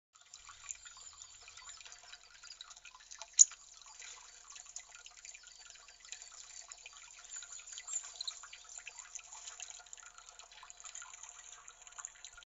2020/05/02 カワラヒワさんズッコケた！
カワラヒワさん、水場の石の端で足を滑らせて滑落！ ズッコケました！